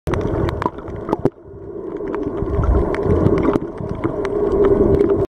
Egypt, Red Sea, Marsa Alam, Diving, November 2017
Прекрасные рыбки в Красном море, Египет.